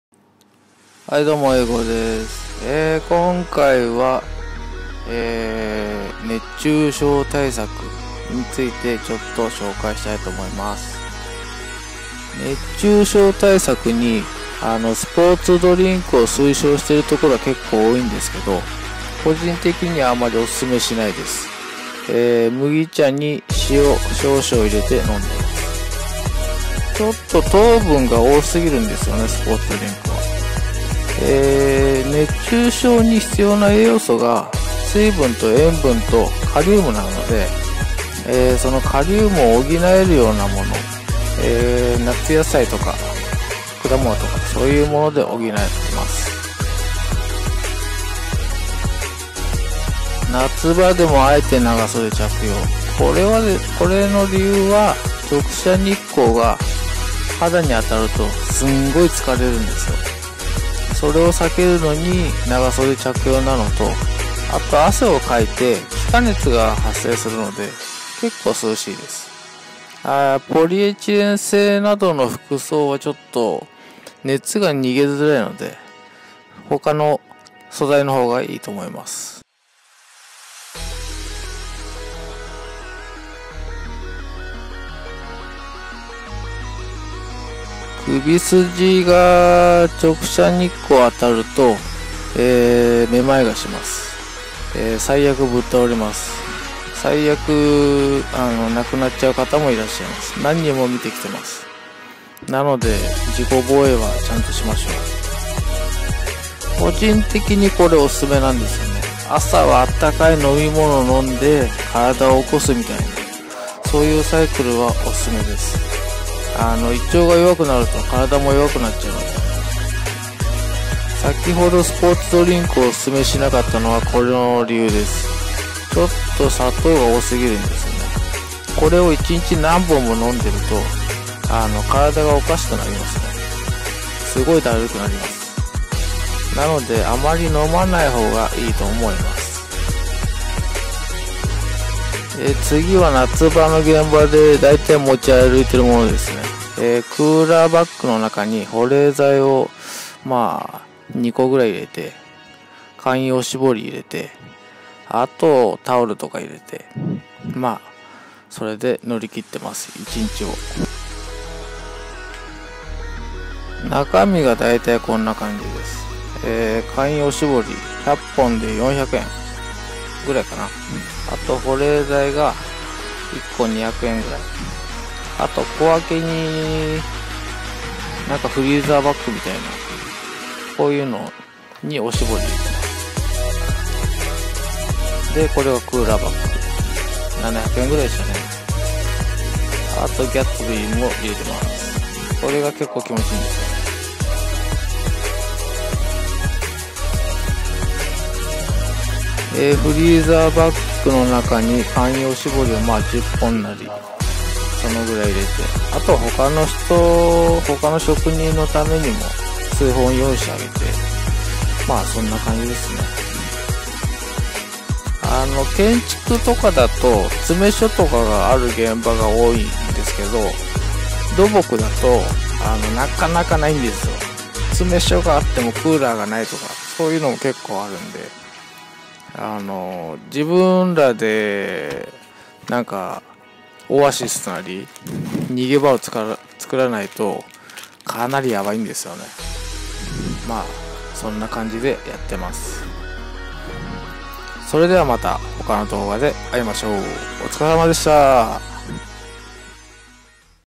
[熱中症対策]土建屋さんの裏技 それほど参考ならない動画68
一般向け～建設業における日々の熱中症対策の紹介になります

建築現場はわりと快適な設備が整ってる所が多いですが

建設現場は基本地獄です

時間制限のある
道路工事における上下水道工事、舗装工事ともなれば逃げ場なんてどこにもありません

自己防衛の参考になれば幸いです(*´ω｀*)

高評価！低評価！どちらでもおっけーです！